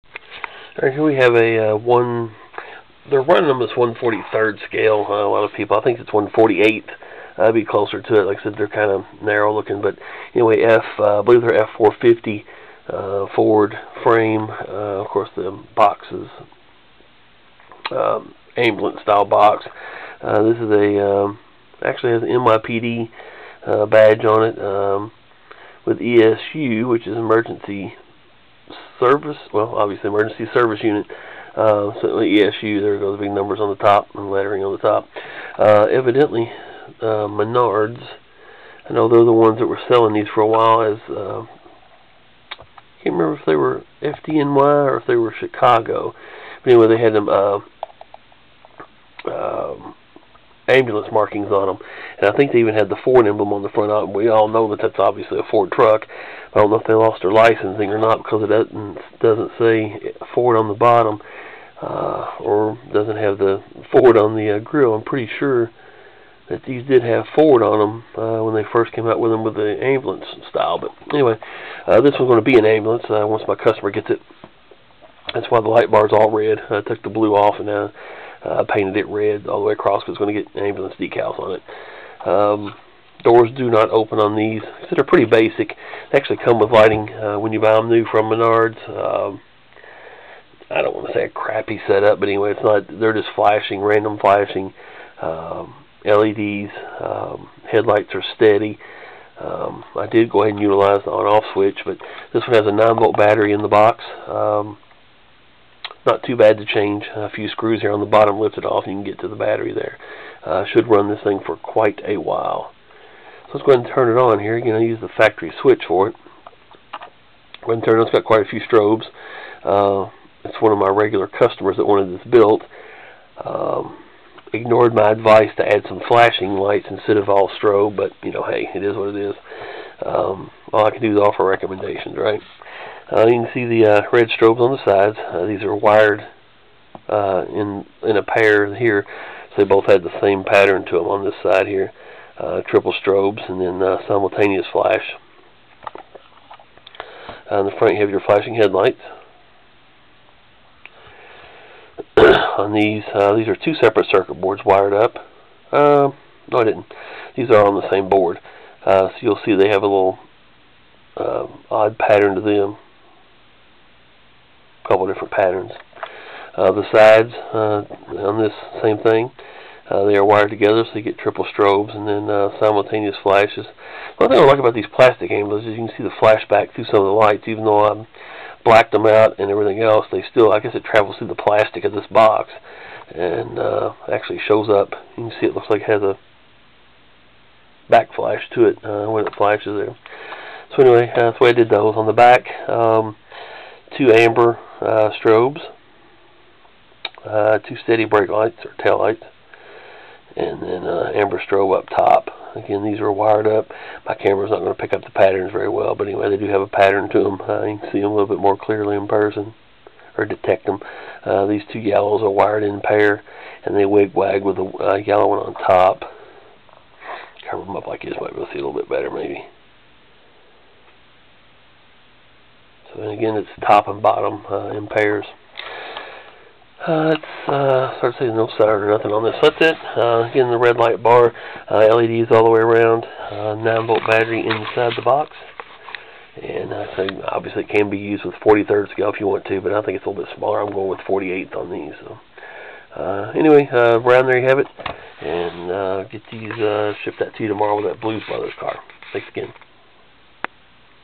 Custom 1:48 Ford F-450 NYPD ESU diecast model with working lights
Here is one of those trucks that Menards sells.  This one has the NYPD Emergency Services Unit decals on it but they will be changed out to decals of an EMS company.  No idea which one since I'm not doing the decals.  Ripped out the weak factory lighting out of this and added some circuit board controlled LEDs.  I suggested some flashing LEDs for the box to help break up the strobes a little bit but the customer wanted all strobes.  Well, here you go!